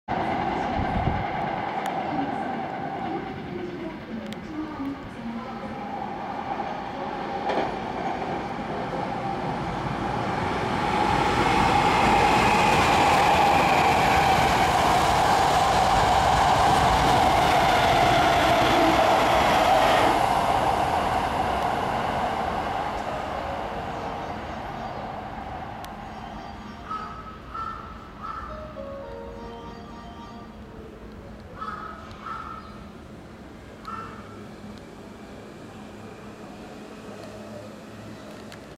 阪急京都線上新庄駅を通過する8300系8315Fの特急梅田行き
2018年10月19日（金）撮影。阪急京都線上新庄駅で撮影した、8300系8315Fの特急梅田行きの通過シーンです。